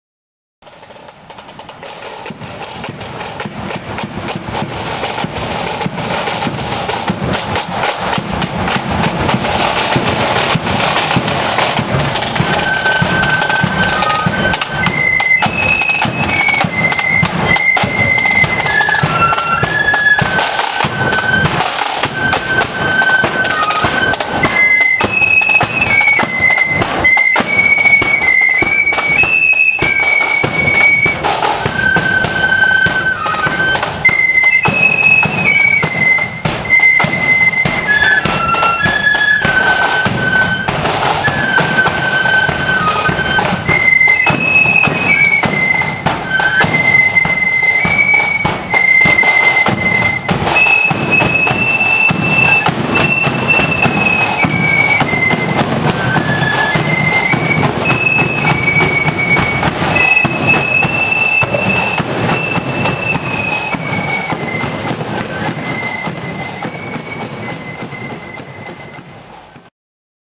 South Down Defenders @ Lambeg Orange & Blue Parade 2015
South Down Defenders on the march in Lisburn for Lambeg Orange & Blue annual Band Parade  Saturday 22/08/2015